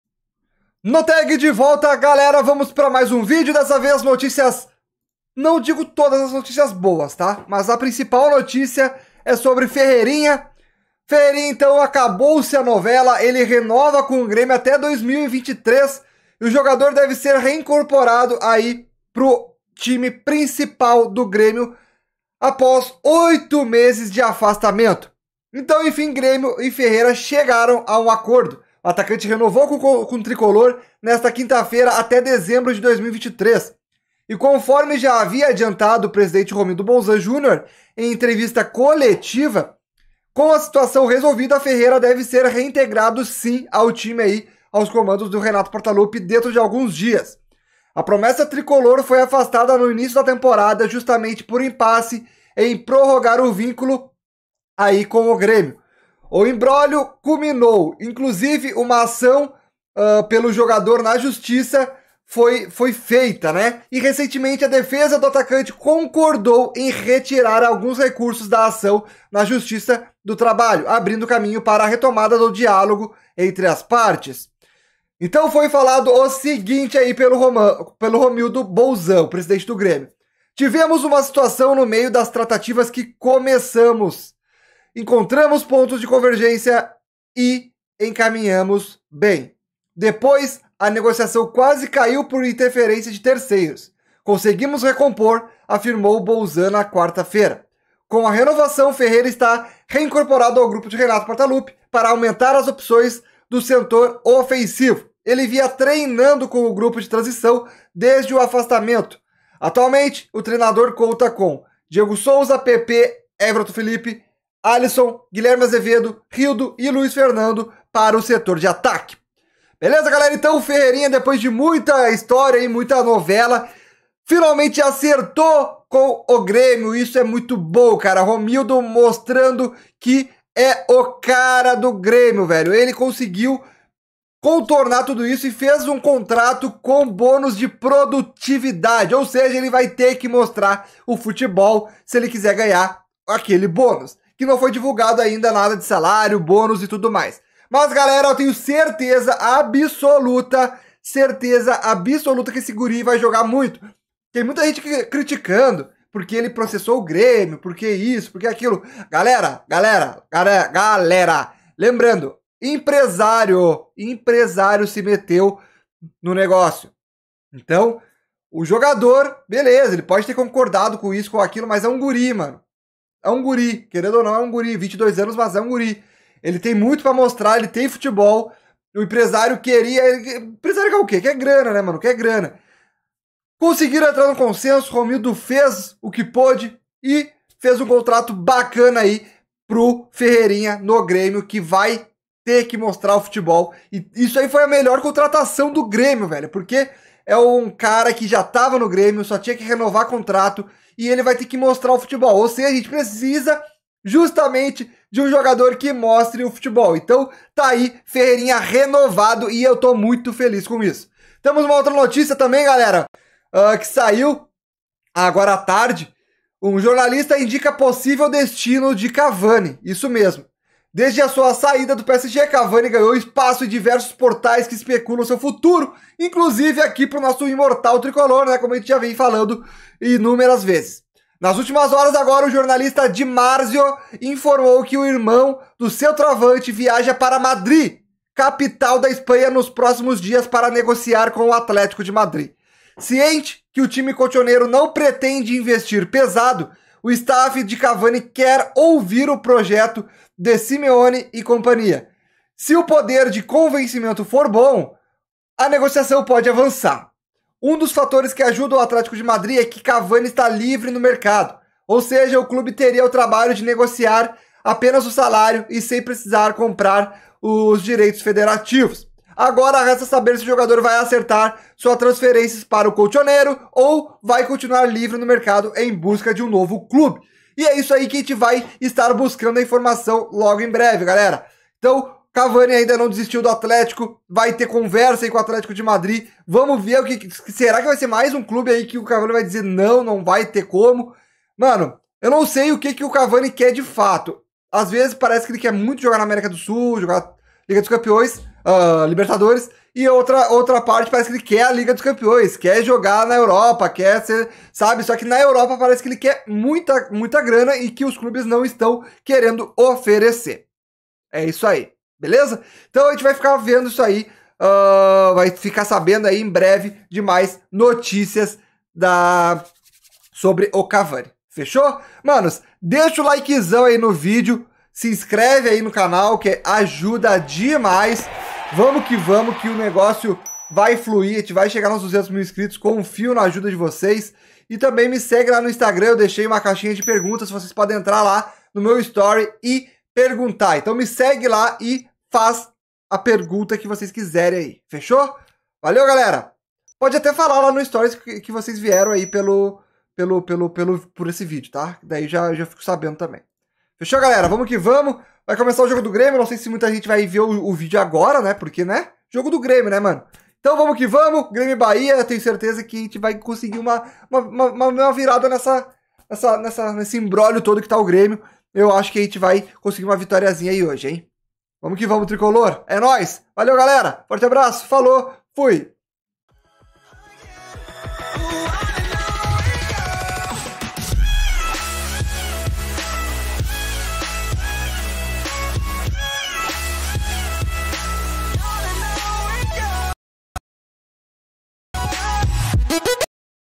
CAVANI PRÓXIMO DE DECIDIR ONDE JOGARÁ | FERREIRINHA RENOVA ATÉ 2023 COM O GRÊMIO!
Ferreirinha tem um contrato renovado com bônus por produtividade e Cavani pode estar próximo de acerto com clube!